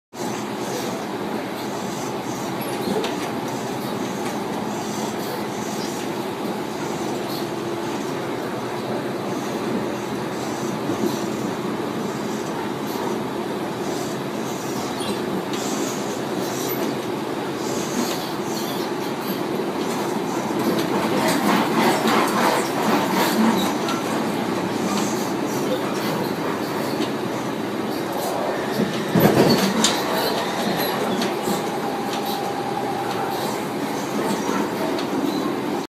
Gangway/inter Carriage Doorway Fail
I am surprised to say the least that this is even allowed to be in service!! This Tangara set is dirty, graffiti around it!!